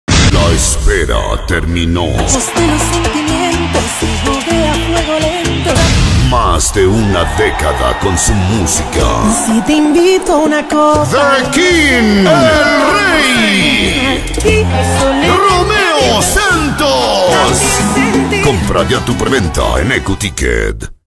PROMO - Romeo Santos en Guayaquil Ecuador 2015 - El Rey de la Bachata
Se viene el mejor concierto de la Bachata en Guayaquil junto al Rey ROMEO SANTOS los esperamos compra ya tus entradas en todos los puntos de ventas de Tickeshow